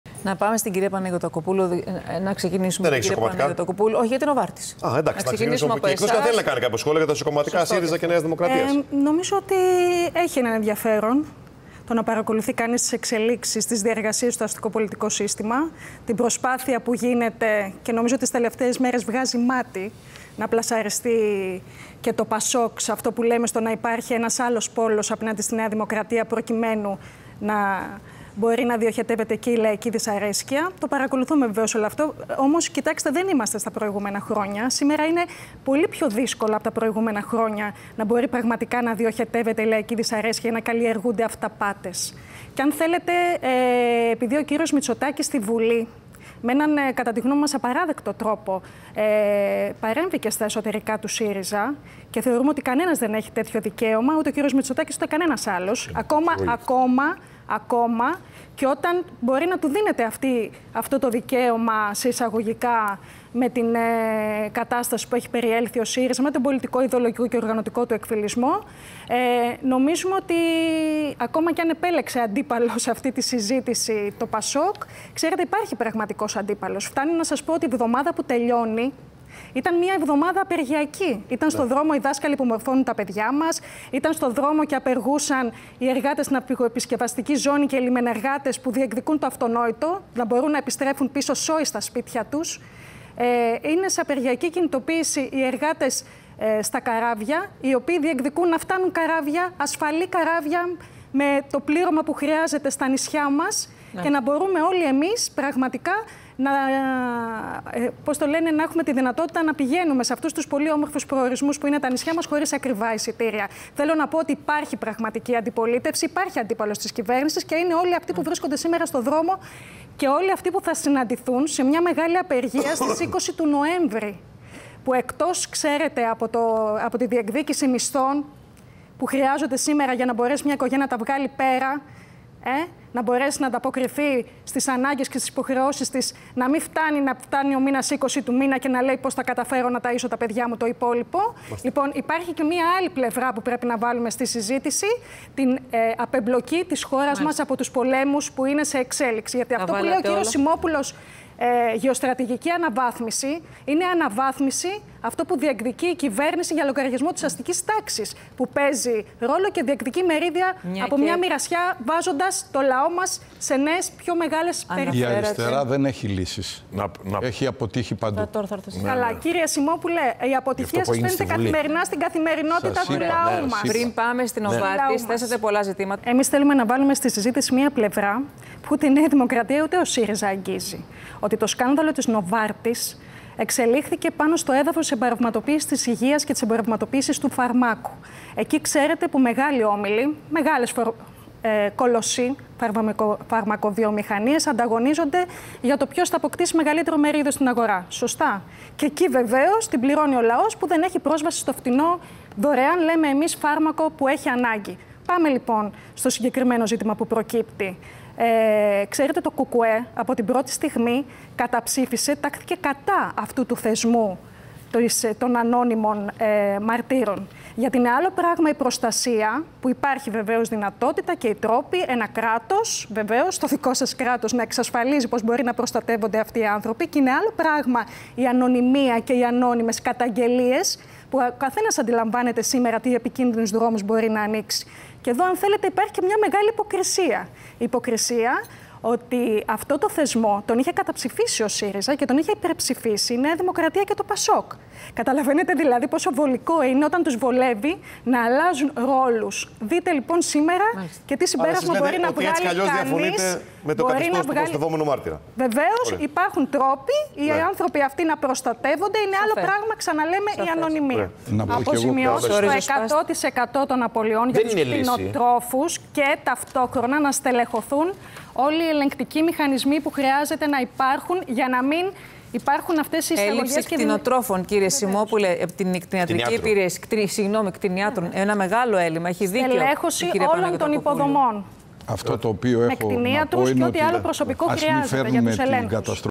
Χρ. Παναγιωτακοπούλου: Πραγματικός αντίπαλος της κυβέρνησης είναι όσοι βρίσκονται στο δρόμο
«Βγάζει μάτι» τις τελευταίες μέρες η προσπάθεια που γίνεται να πλασαριστεί το ΠΑΣΟΚ ως ένας άλλος πόλος απέναντι στη ΝΔ, «προκειμένου να μπορεί να διοχετεύεται η λαϊκή δυσαρέσκεια», τόνισε η Χριστίνα Παναγιωτακοπούλου, μέλος της ΚΕ του ΚΚΕ σχολιάζοντας τις διεργασίες στο αστικό πολιτικό σύστημα.